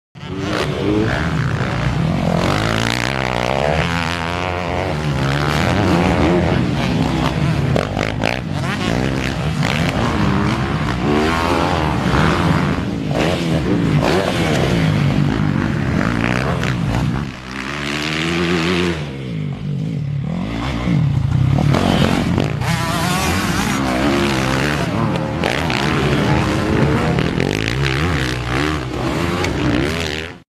Another day in the office doing motos at ClubMX with Jeremy Martin
Catch up with Jeremy Martin on the #6 Muc-off / FXR / ClubMX Yamaha race team.
Another training day at the office in South Carolina preparing for the 2023 monster energy supercross rounds.